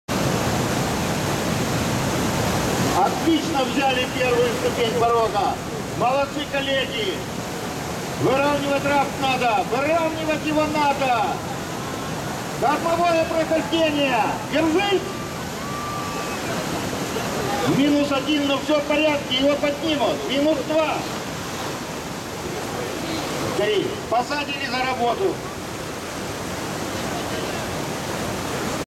IRAN Rafting team,Russia 2011,May Belaya river
IRAN Rafting team,Russia 2011,May with captain Payam Aslani in Belaya river